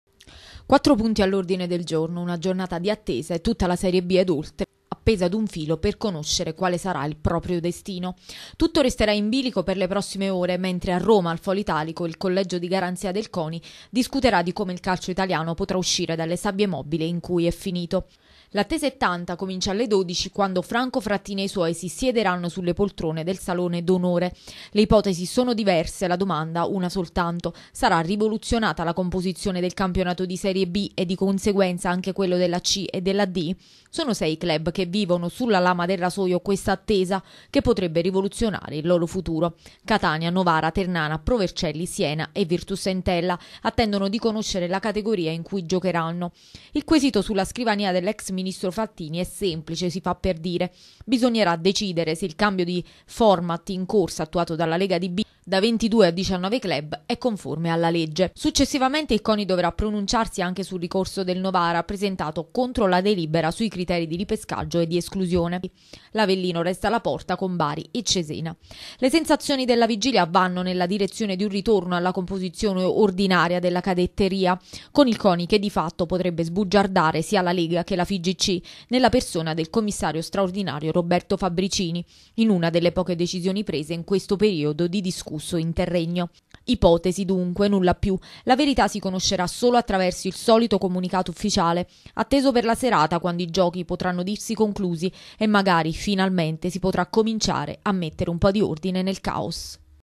La palla torna al Coni, domani il giorno della verità
Il ministro Frattini sarà chiamato a discutere sulla legittimità della B a 19 squadre e sui criteri per i ricorsi. Alle 12 si comincia, la decisione attesa in serata